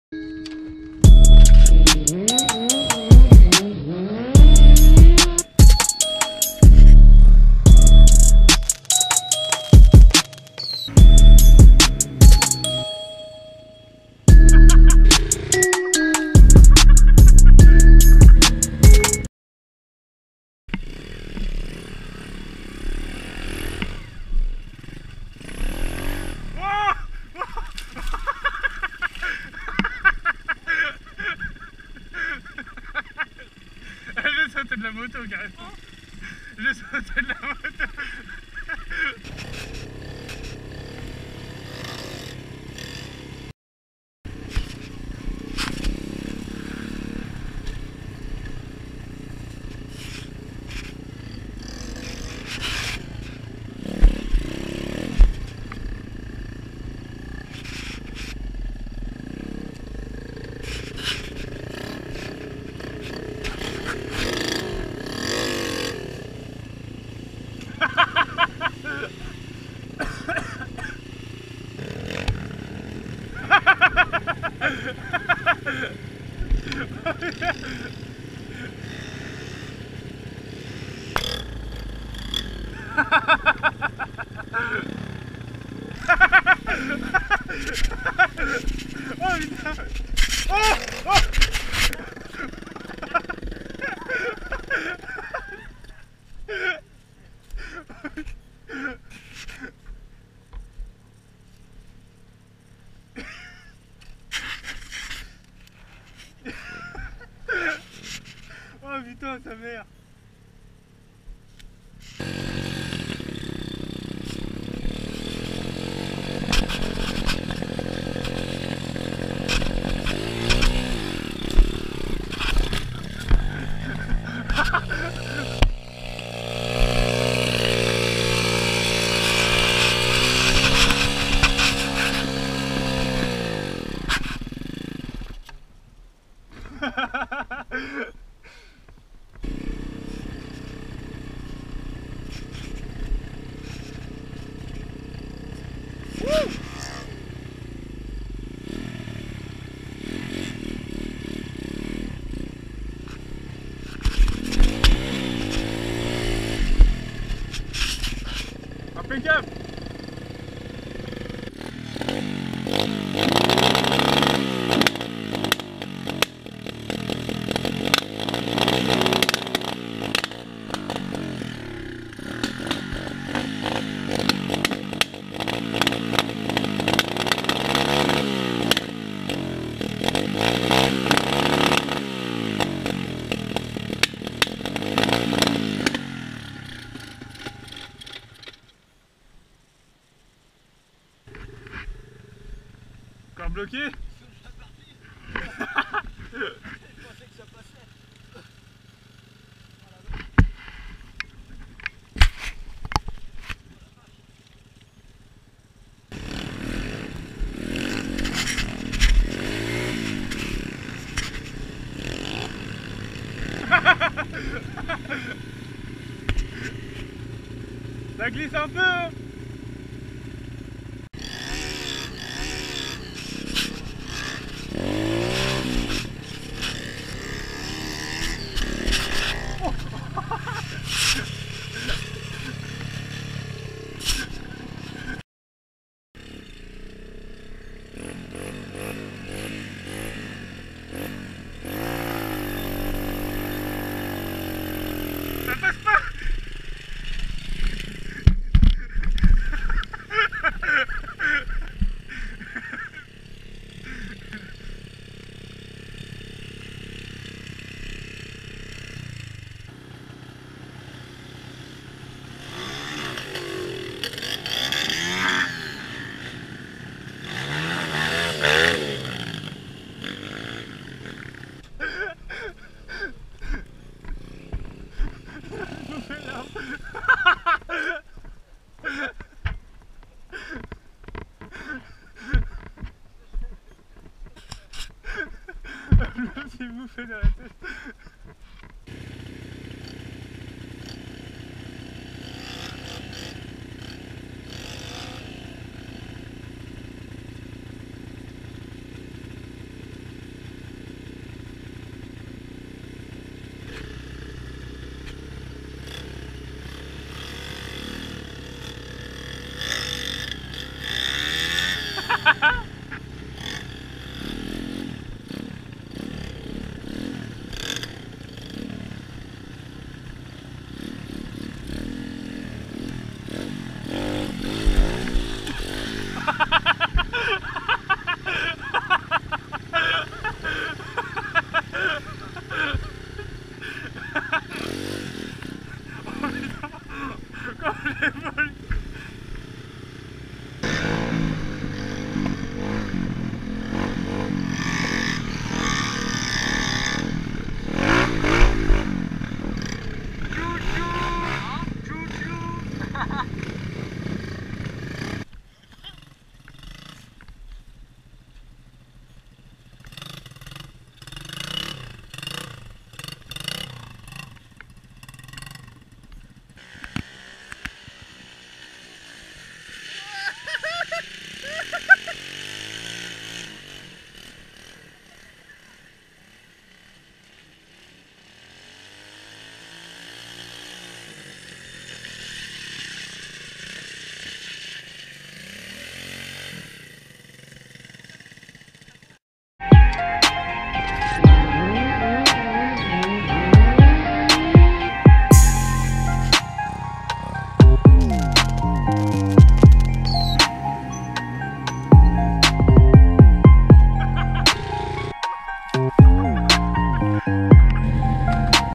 DirtBike 125cc BEST OF / Crash&FouRire !!
Yop !! Simple vidéo , une compilation de plusieurs sortie filmé !
Je vais essayé de faire beaucoup plus de contenus cette année du stunt de la dirt et autre

Hésité pas a laisser un commentaire et à vous abonné